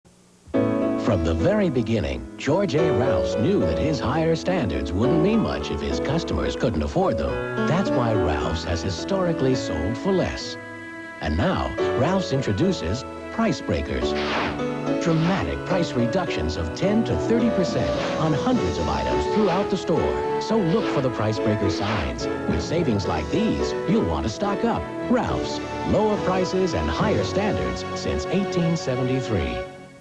1989 Ralph's Grocery Store TV Commerical
The commercial was originally aired in May 19, 1989. It was shown during an airing of the Disney live action folm "Worlds Greatest Athlete" on Channel 5 KTLA. KTLA was an independent station.